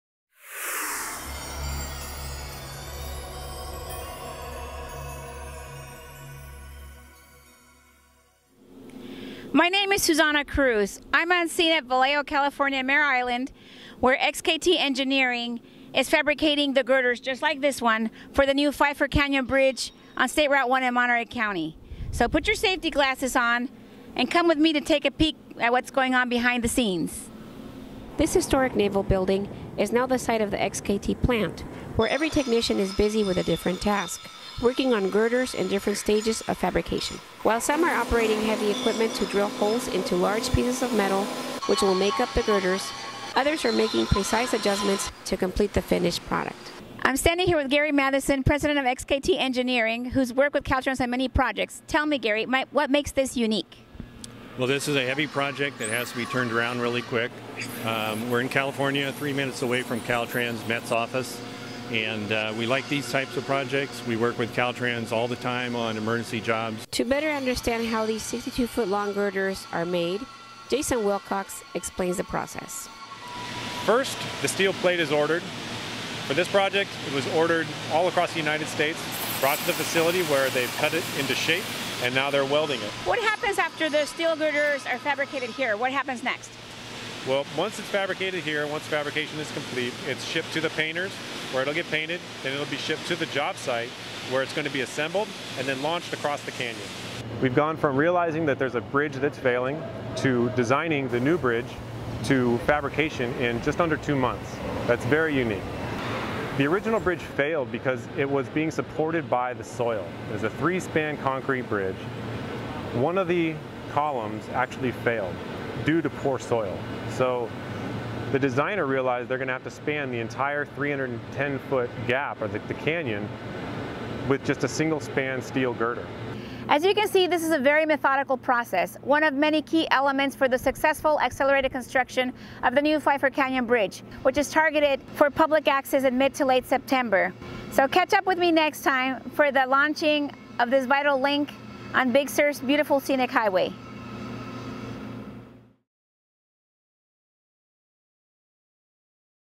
New Pfeiffer Canyon Bridge Girders - Caltrans News Flash #133
In this Caltrans News Flash, District 5 Public Information Officer Susana Cruz takes you behind the scenes at the XKT Engineering Plant in Vallejo, CA, where bridge parts called “girders” are currently being fabricated for the new Pfeiffer Canyon Bridge on State Route 1 in Monterey County.  This video provides an opportunity to see just how these large steel plate girders are made as Gary Mathison, President of XKT engineering and Caltrans Structural Representative Jason Wilcox explain the process.  Once fabrication is complete, the girders will be trucked to Stockton, CA where they will be painted then transported to Pfeiffer Canyon and assembled on-site and launched across the 310-foot expanse.   Public access for this new single-span steel girder bridge is scheduled for late September 2017.